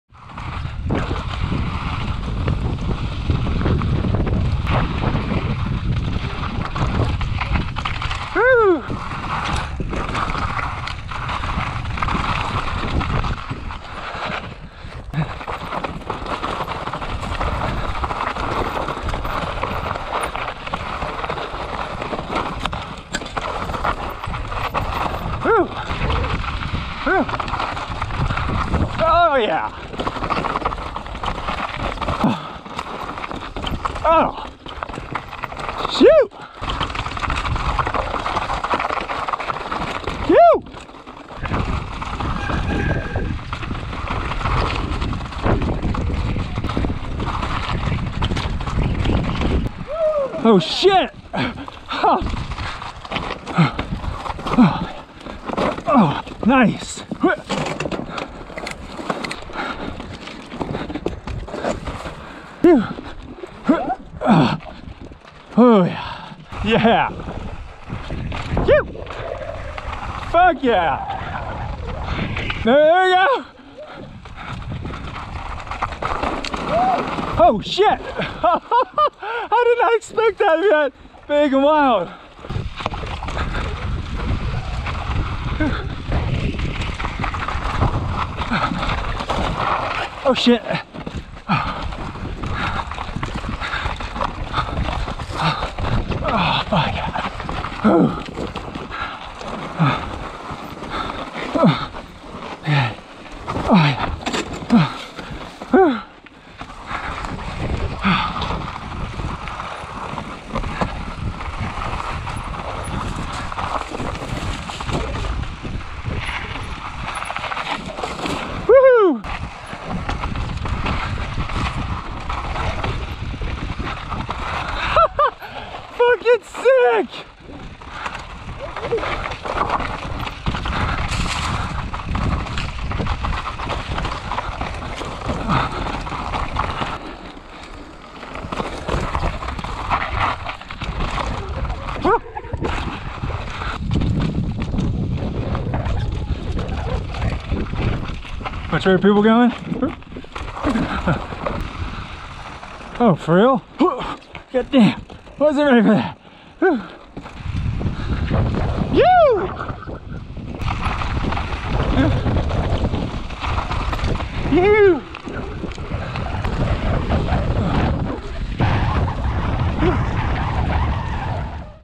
Skyfall | Bass Lake
1 mile 1,200 ft of descending.  It was steep and loose at the top and hectic with rocks and tricky turns at the bottom.  Riding it blind following Tyler Honeyman, would be pretty easy to go over the bars on this one.